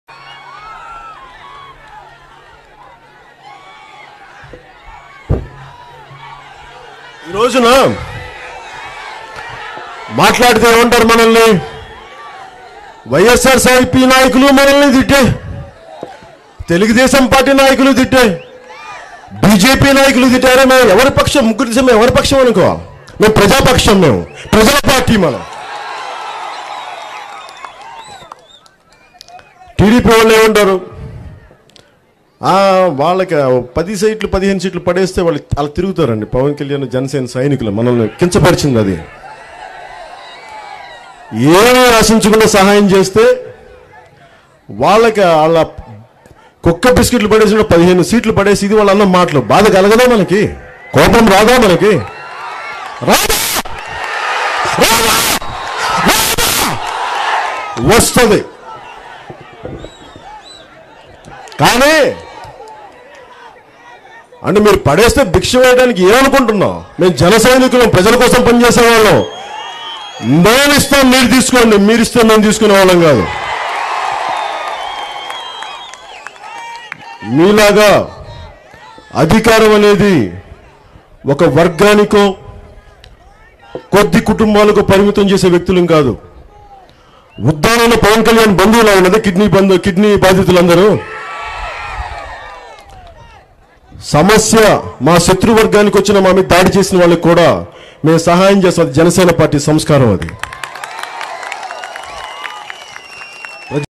టీడీపీ వైస్సార్సీపీ బీజేపీ పై విరుచుకు పడ్డ పవన్ కళ్యణ్ || Tollywood film news
Watch టీడీపీ వైస్సార్సీపీ బీజేపీ పై  విరుచుకు పడ్డ పవన్ కళ్యణ్  || Tollywood film news